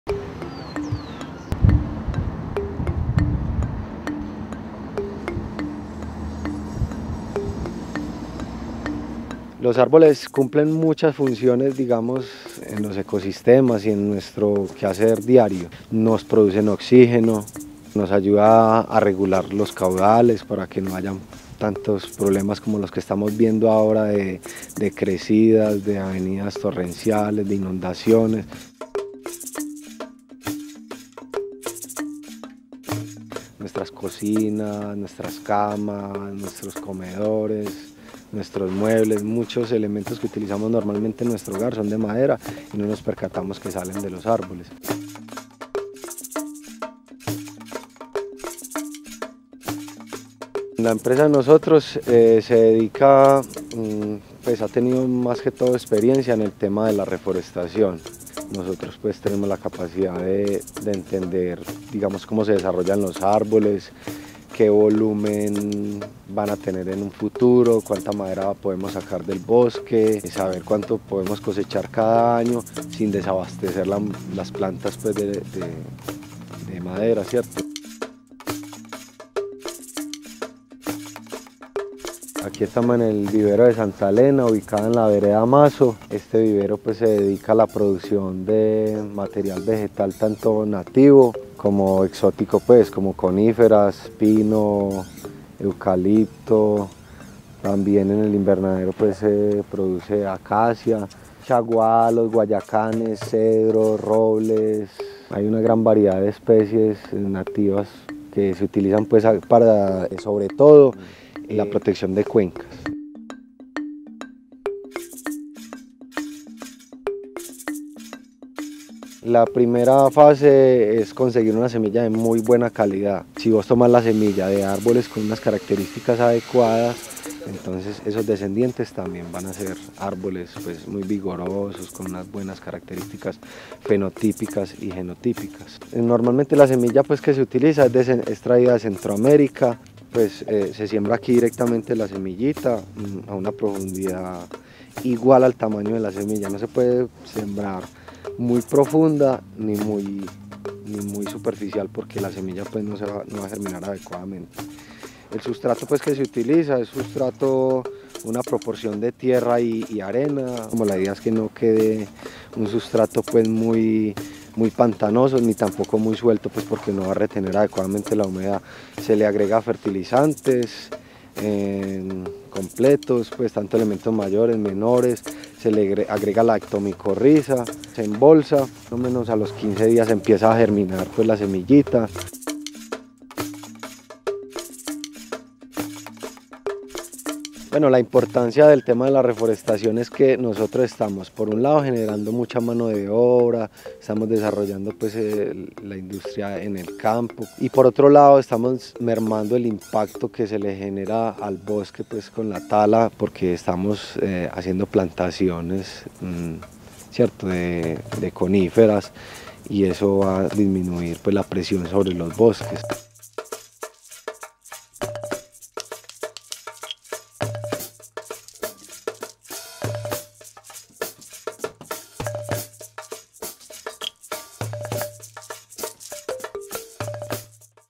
Ingeoforest
Visitamos una empresa encargada de ayudar en la reforestación de distintos tipos de bosques.  Ellos nos mostraron el proceso que seguían las empresas que acudían a ellos desde el sembrado y cómo utilizan la tecnología para hacer este proceso.